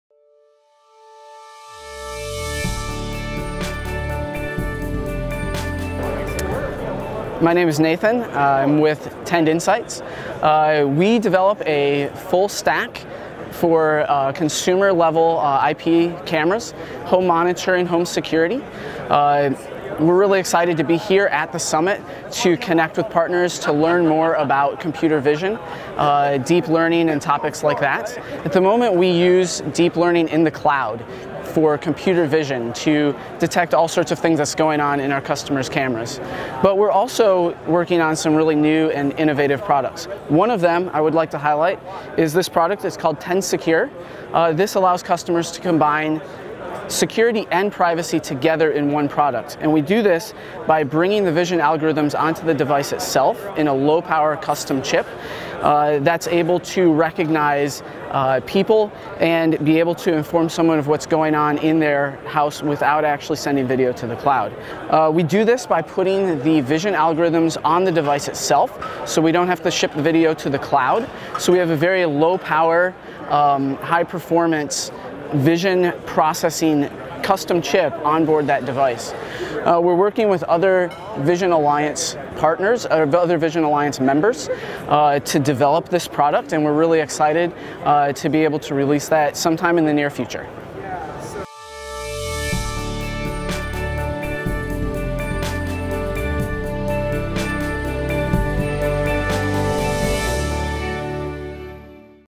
Tend Discussion of its Latest Products and Future Plans
Nathan Kopp, Software Architect at Tend Insights, demonstrates the company's latest embedded vision technologies and products at the May 2016 Embedded Vision Summit. Specifically, Kopp provides a company overview as well as discussing the company's latest Tend Secure product and providing a preview of future plans.

Tend is a maker of lifestyle smart home products and apps that deliver awareness and insights through immersive experiences powered by computer vision technology. As the leading Vision-as-a-Service (VaaS) platform for the connected home ecosystem, Tend offers smart detection, live streaming, cloud video recording and cloud storage services to help major IoT companies bring innovation into their products. Tend supports one of the largest installed bases in vision for the Internet of Things.